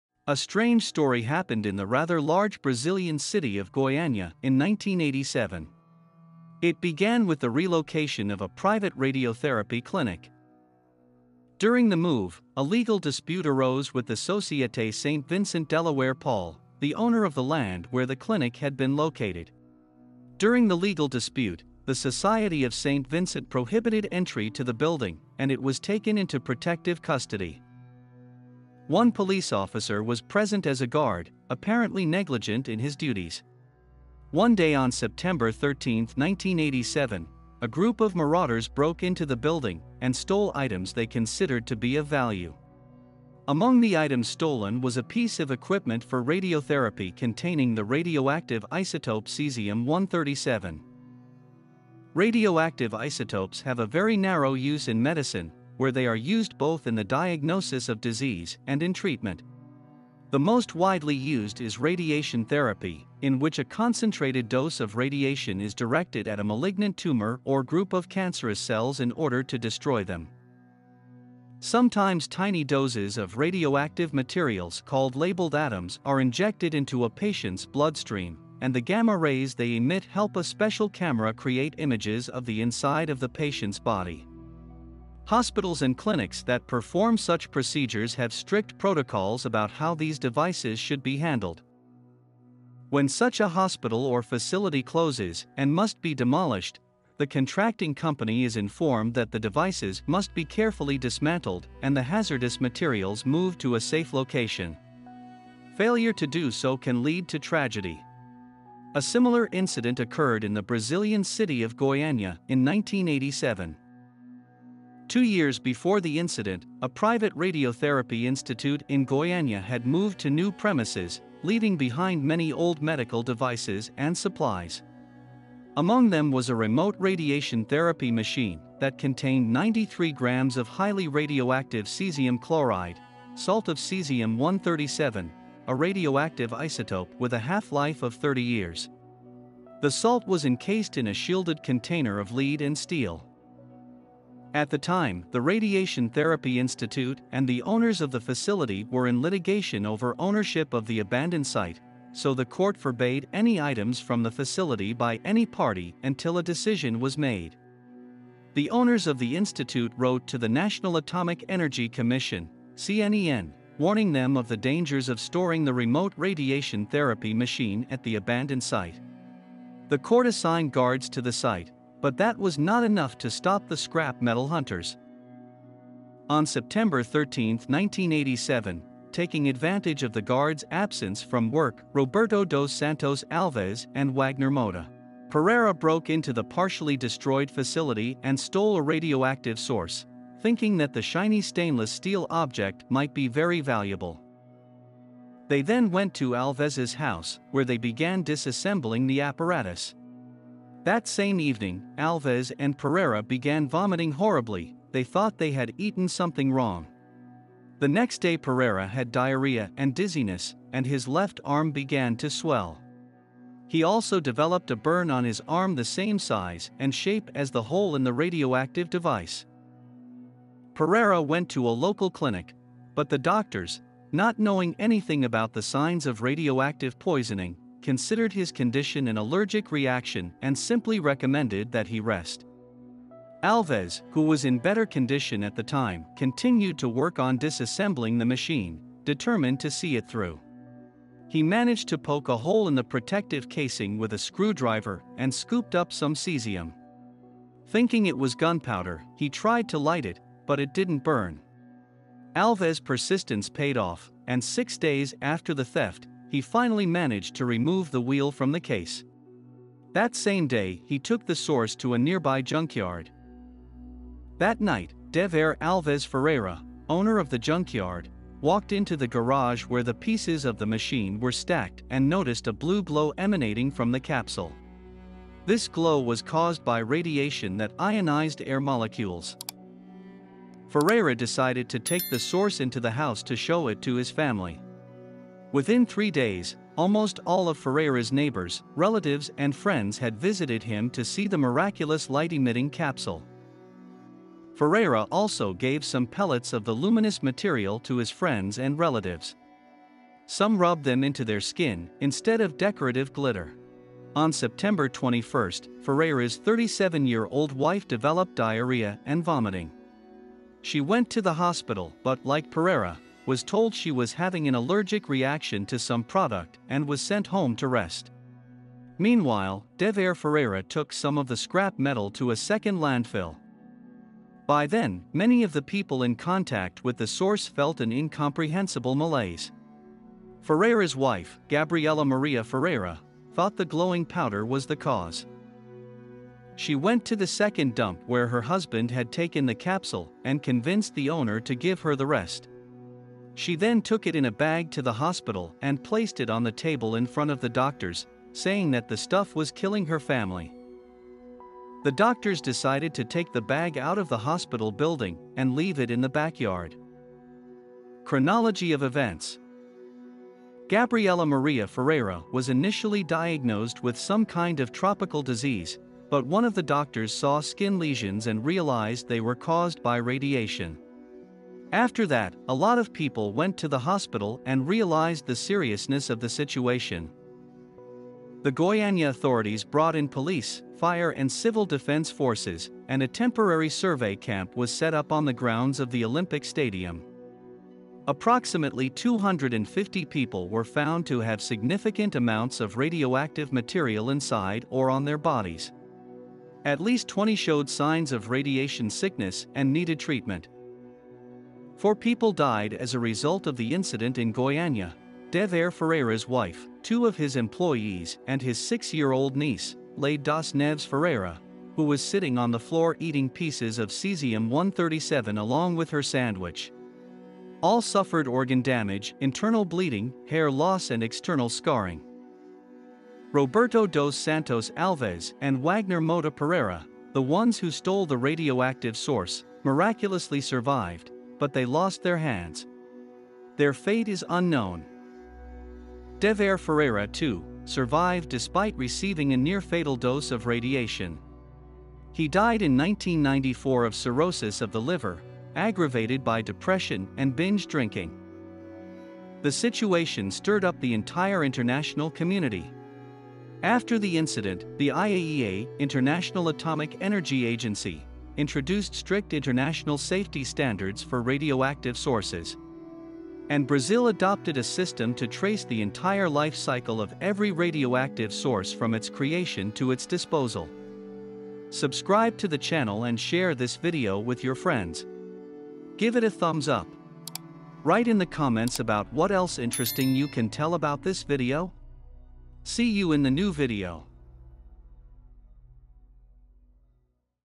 Brazilian Chernobyl. A huge city was exposed to radiation
A strange story happened in a fairly large Brazilian city of Goiania in 1987. It all started with the relocation of one of the private clinics that was engaged in radiotherapy. As a result, a huge city was exposed to radiation